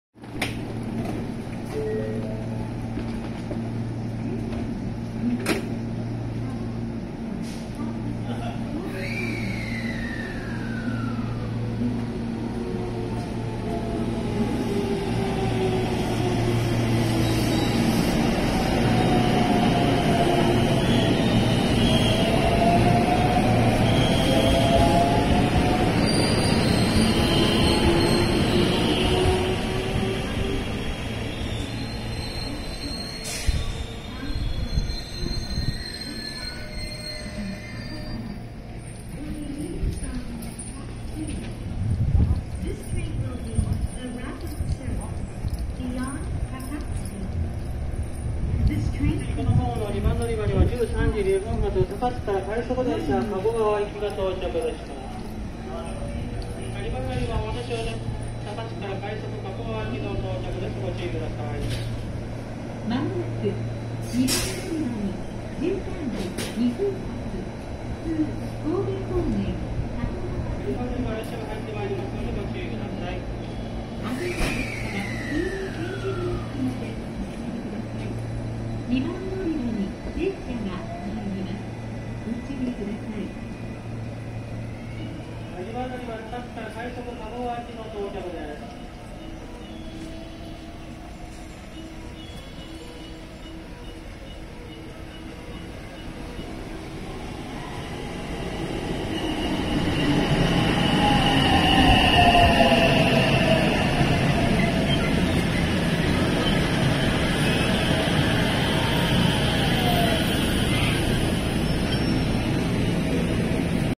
JR西日本 223系6000番台R201編成ダブルパンタ 湖西線 B普通 京都行き 223系2500番台 R51編成 湖西線 B普通 近江舞子行き 225系100番台U5編成 普通 加古川行き 山科駅
JR西日本

223系6000番台R201編成ダブルパンタ
湖西線 B普通 京都行き

223系2500番台 R51編成
湖西線 B普通 近江舞子行き

225系100番台 U5編成＋223系2000番台 V編成
普通 加古川行き

山科駅
20230915